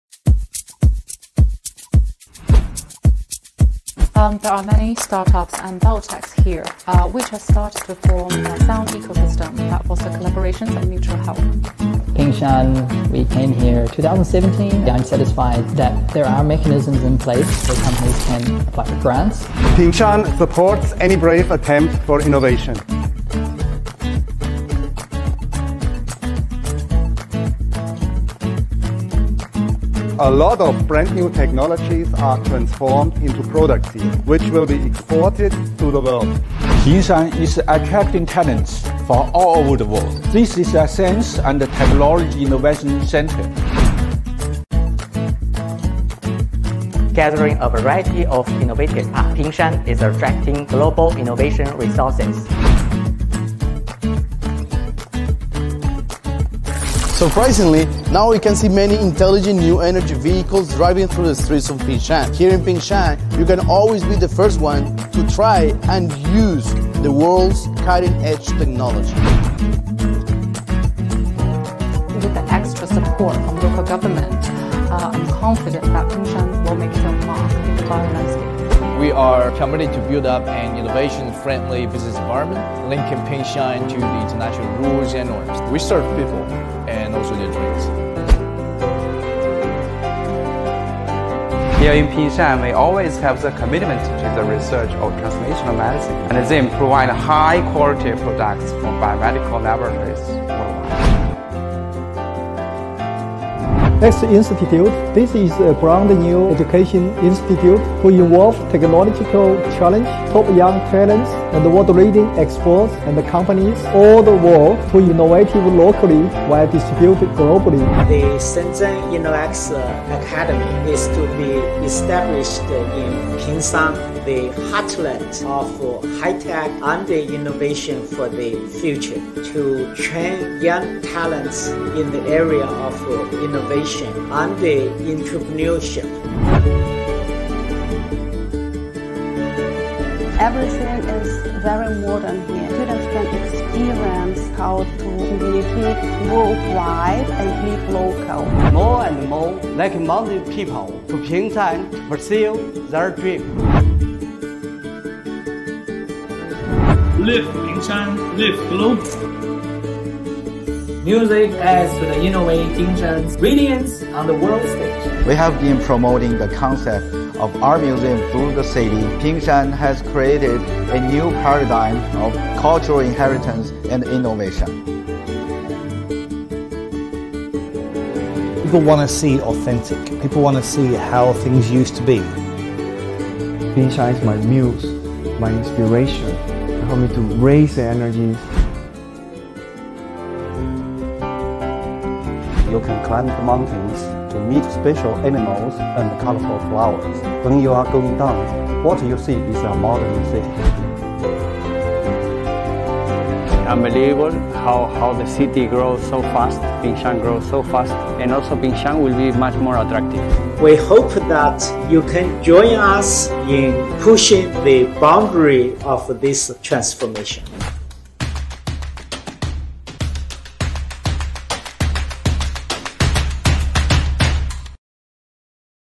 Global promotional video of innovative Pingshan
This is vibrant Pingshan — a district developing with advanced technologies, brilliant innovation, emerging industries, smart brains, rich culture, beautiful ecological environment and a bright future.